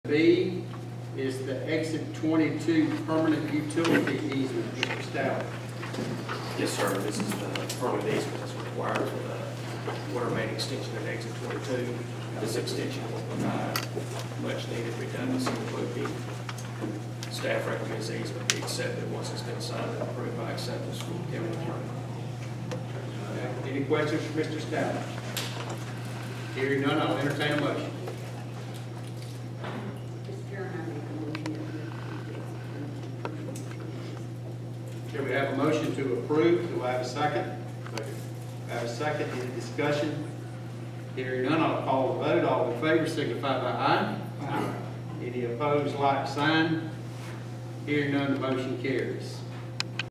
7 b. Exit 22 Permanent Utility Easement @ LCC Regular - 2022-04-26
Lowndes County Board of Commissioners (LCC)  Regular Session - April 26, 2022, Valdosta, Lowndes County, Georgia.